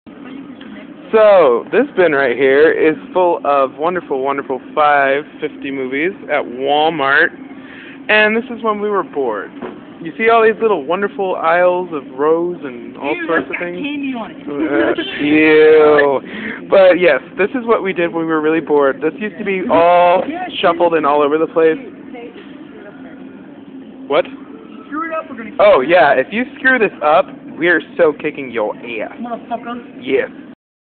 Wal-Mart $5.50 Bin...
so yeah, it was like one-three in the morning and we were bored and trying to find movies we wanted down in the $5.50 bin and we figured, what's the best way to find good movies... sort them out...